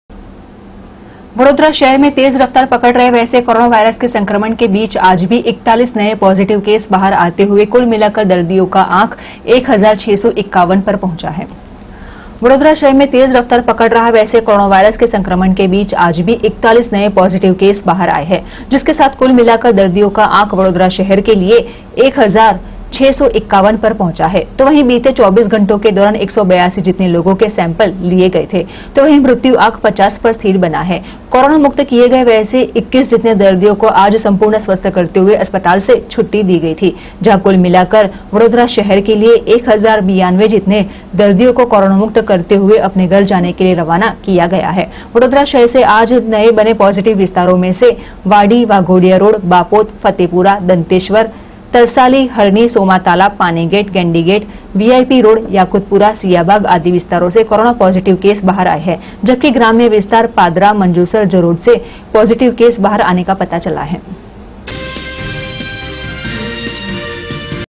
Ibn news vadodara
वडोदरा शहर में आज भी 41 नए पॉजिटिव केस में बढ़ावा, कुल आक 1651 पर